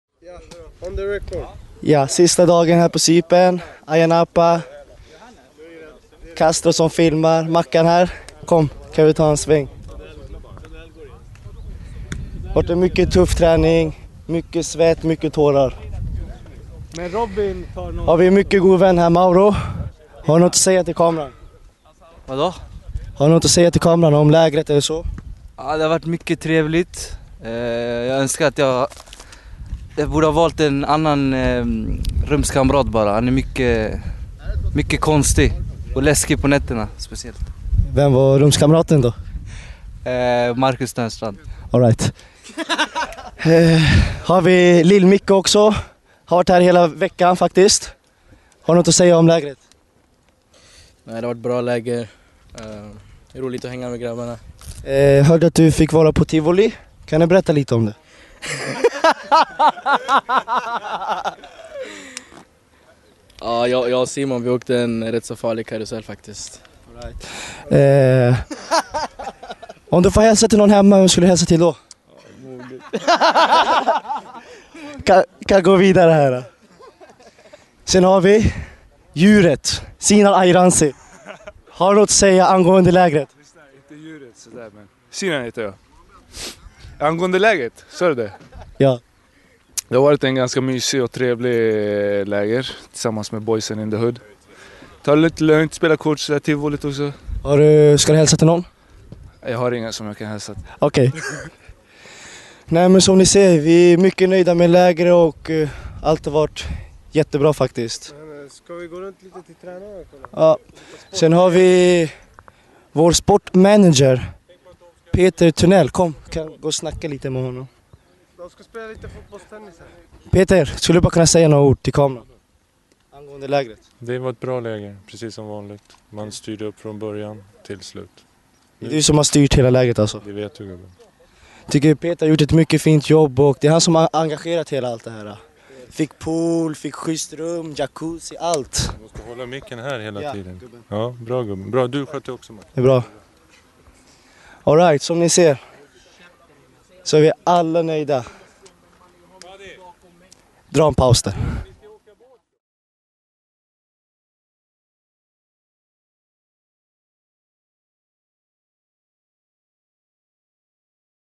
HTV: Så upplevde spelarna Cypernlägret - med stjärnreporter Marcus Törnstrand
Hur har Bajenspelarna haft det under veckan på Cypern? Det nybildade HTV-teamet Marcus Törnstrand och Sebastian Castro-Tello söker svaret på frågan.